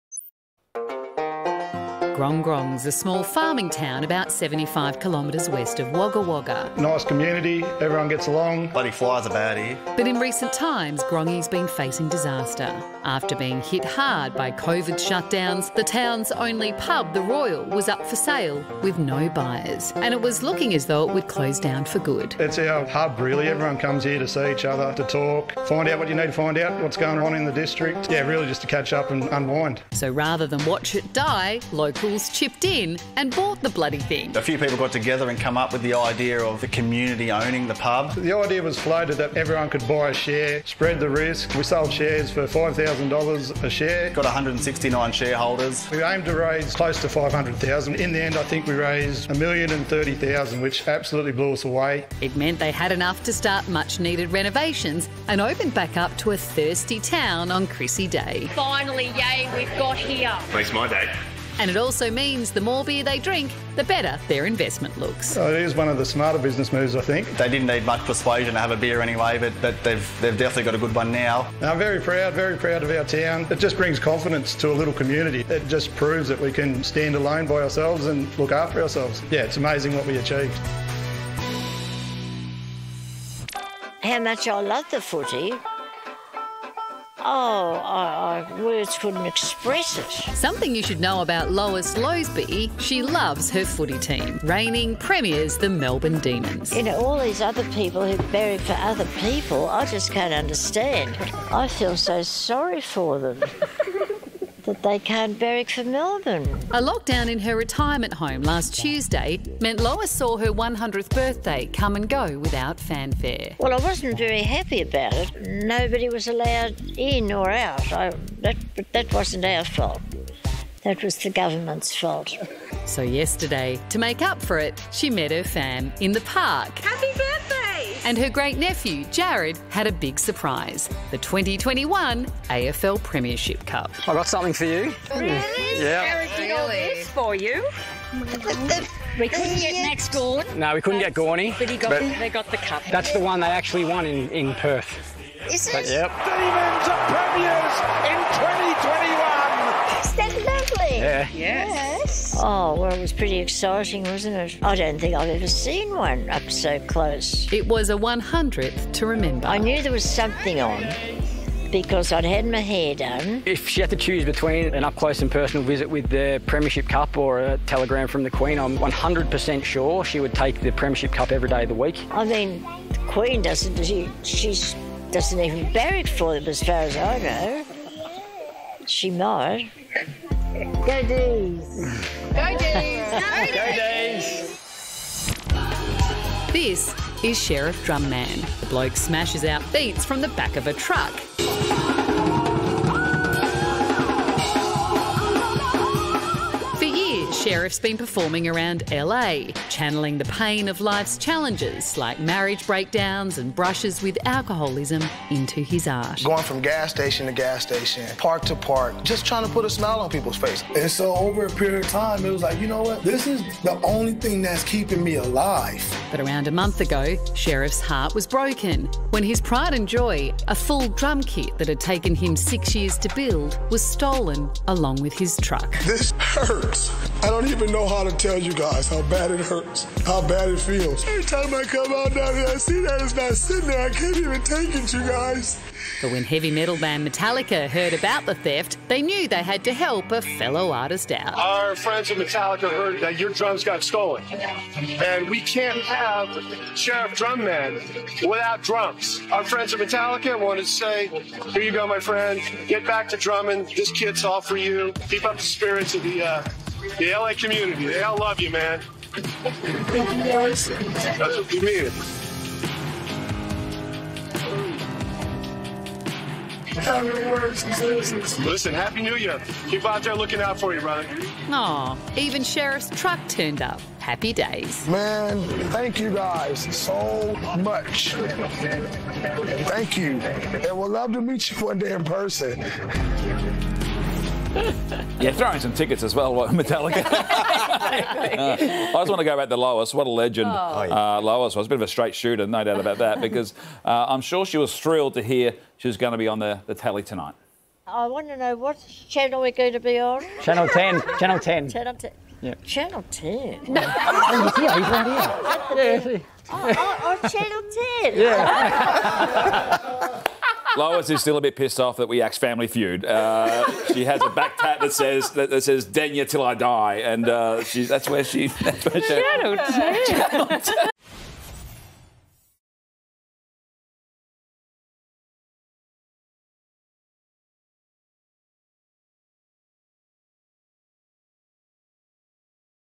Australian Town Grong Grong Bands Together To Save Local Pub
Having been hit hard by Covid, a local pub was set to close for good after no buyers were interested… until the entire town of Grong Grong in NSW stepped in.